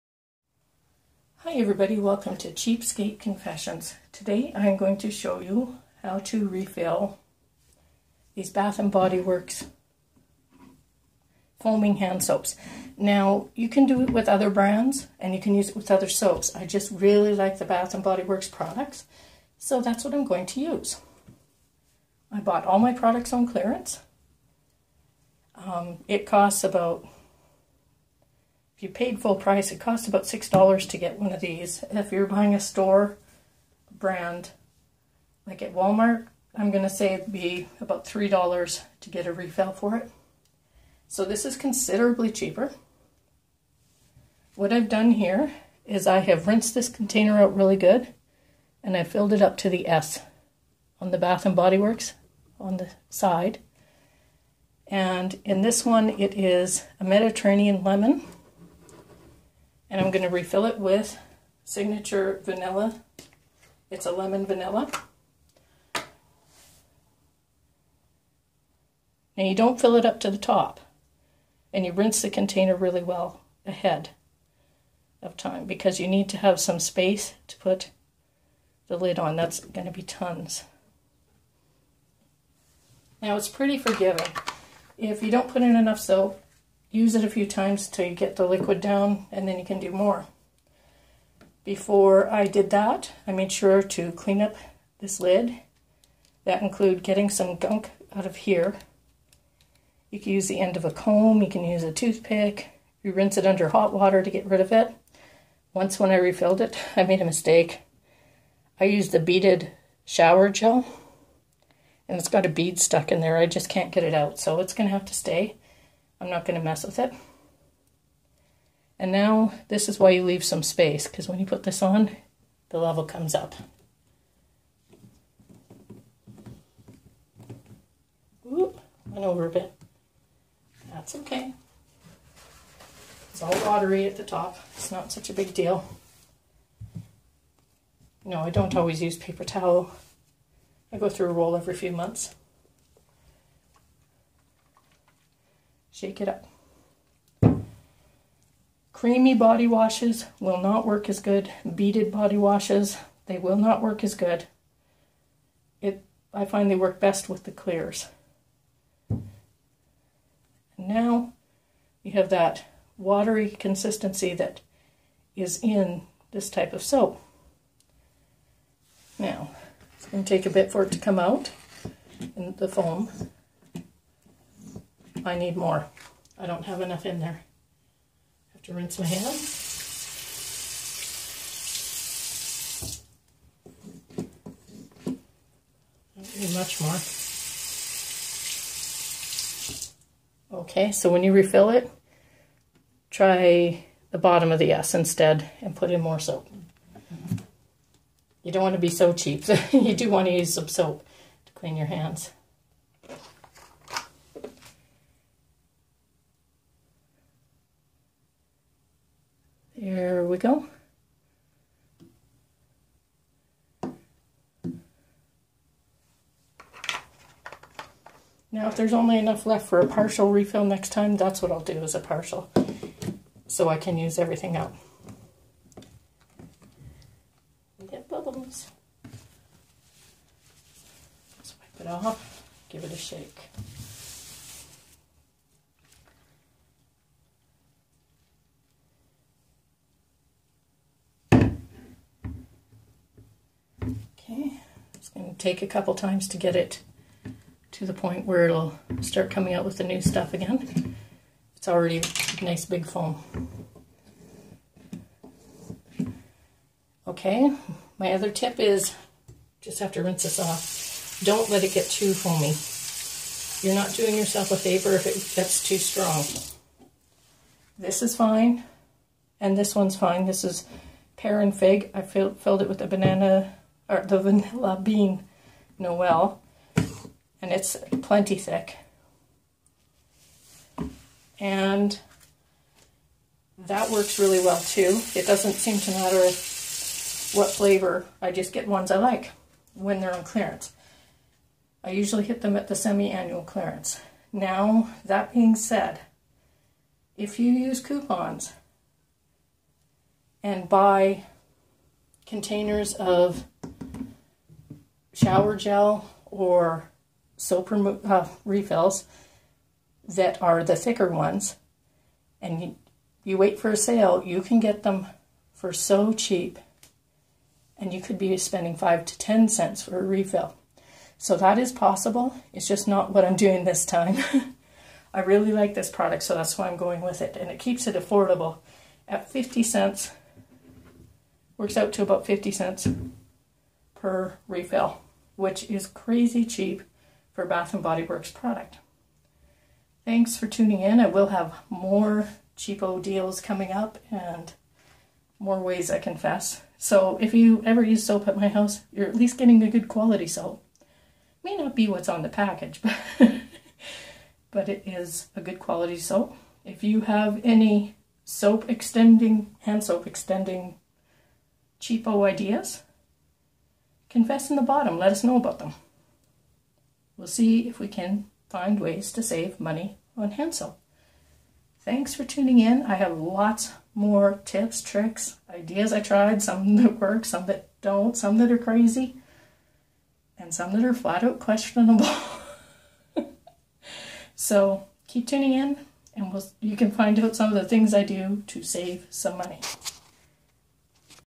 Cheap foaming hand soap refills
Things may not be as the appear to be in my place! My hands are SO clean right now! I want to have fantastic quality for pennies on the dollar. Instead of spending $3 a bottle on a big box store brand product I get 6 months of high quality product for that $3. This tip saves me $5 a month or $60 a year plus tax. Wait and see, these tips add up quickly to HUGE savings.